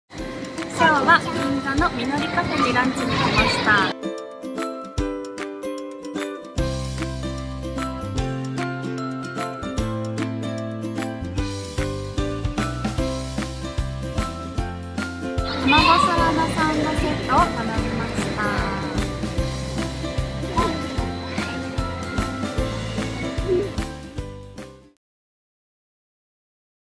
筧さなえさん｜みのりカフェ＠銀座三越｜女子グルメ
グルモ：筧さなえさん
本日のお店：みのりカフェ＠銀座三越
詳細はコチラ